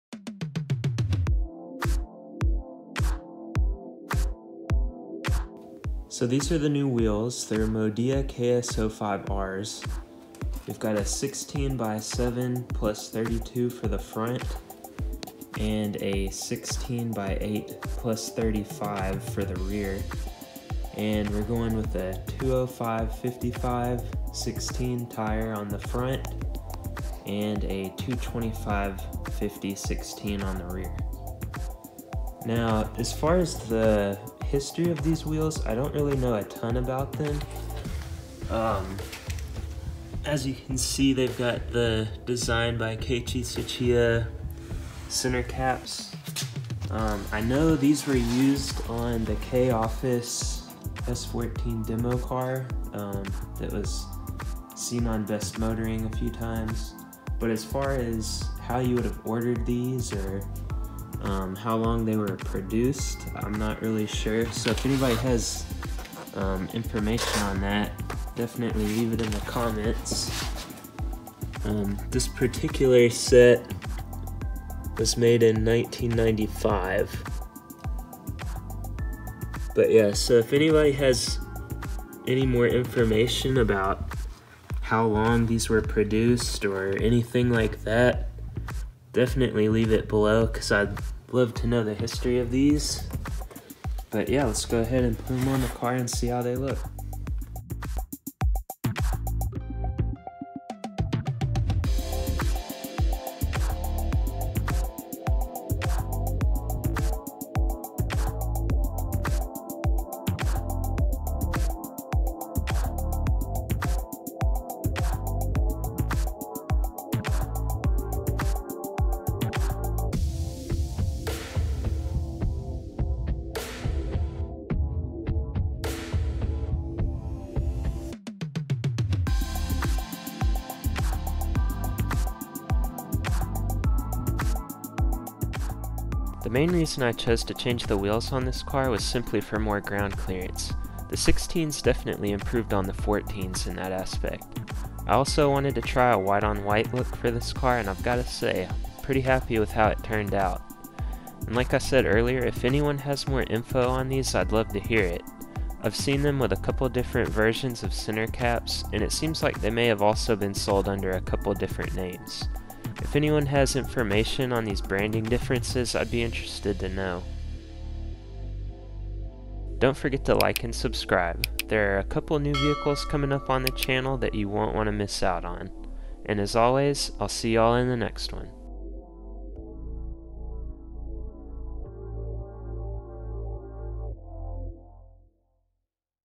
New Wheels on the 240SX | Modia KS-05R
In this video we install a set of Modia KS05 wheels and Hankook Ventus V2 Concept2 tires on the 1993 Nissan 240SX. Wheel and tire specs are 16x7 +32 with 205/55r16 on the front, and 16x8 +35 with 225/50r16 on the rear.